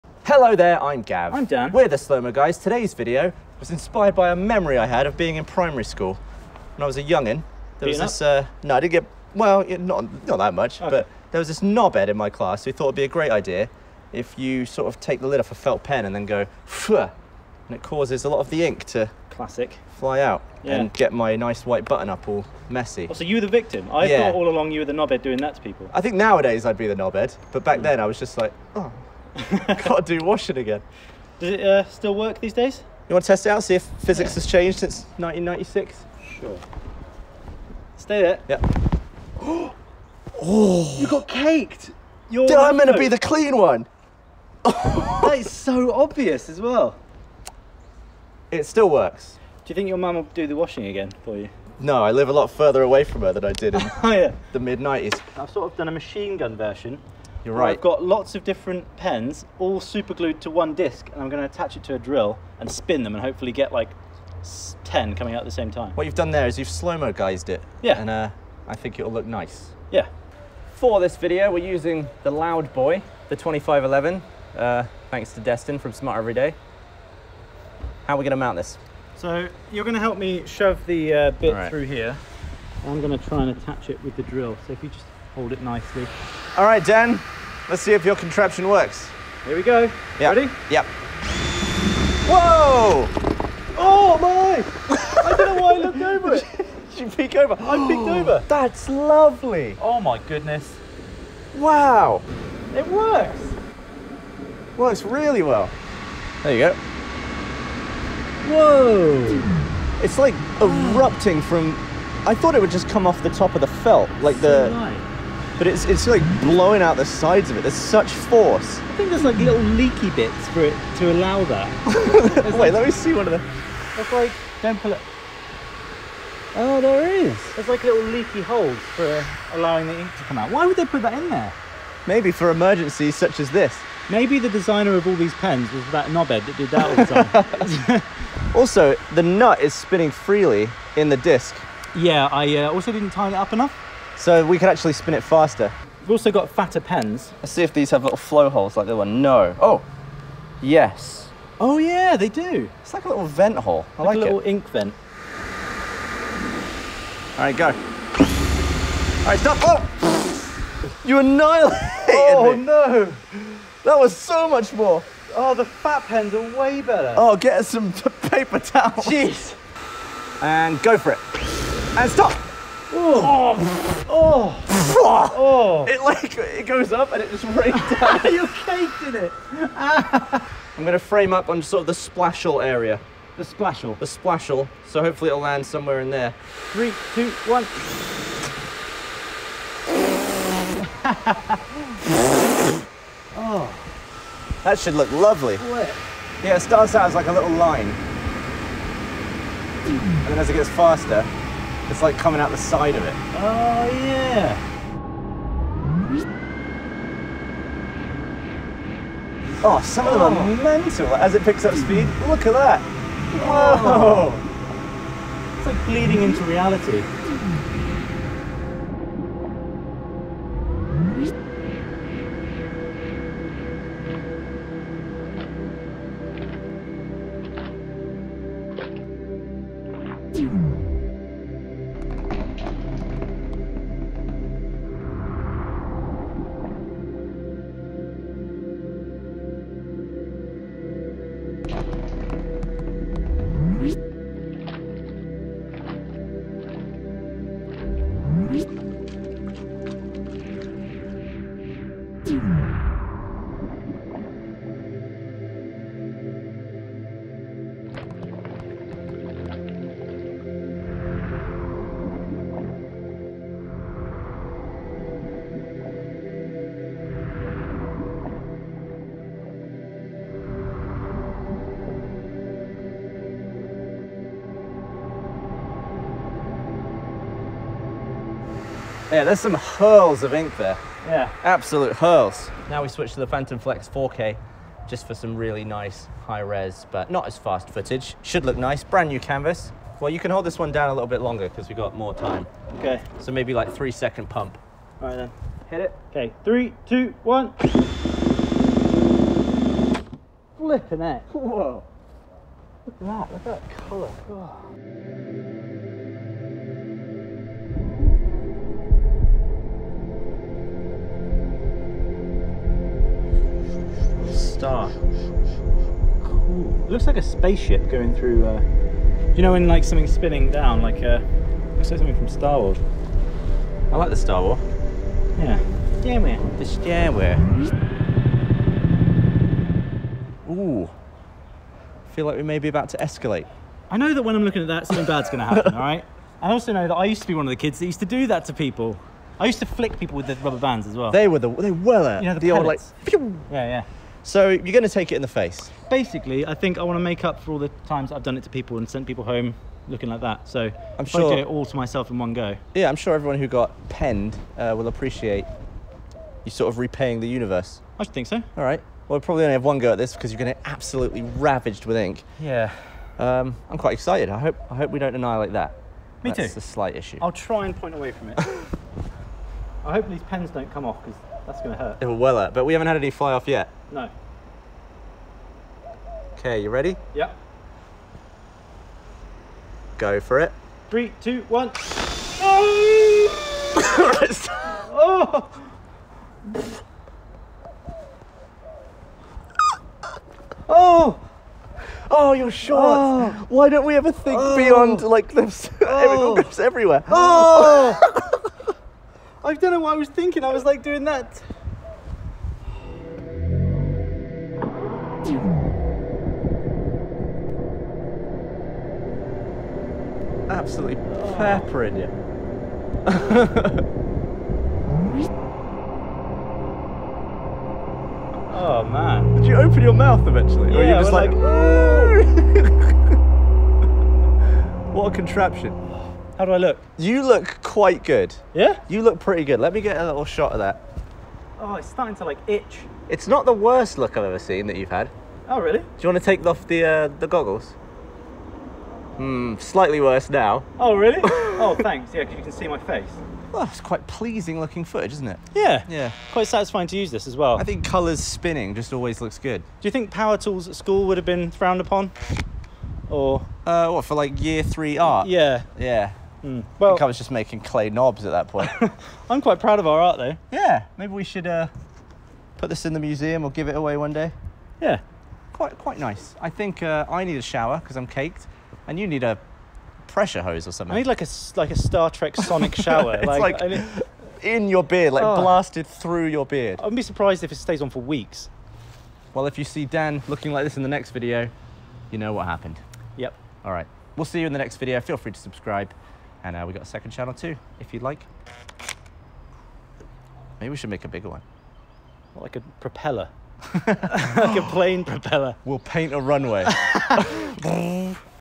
Spinning Ink out of Pens in Slow Motion - The Slow Mo Guys
Using the scientific force known as "the spinnies," Gav and Dan make some splatter paintings using a drill. Gav asked Dan to get some cheap, knock-off felt pens, but he went full Crayola.
Filmed at 1000fps and 28,500fps on the Phantom Flex 4k and the Phantom V2511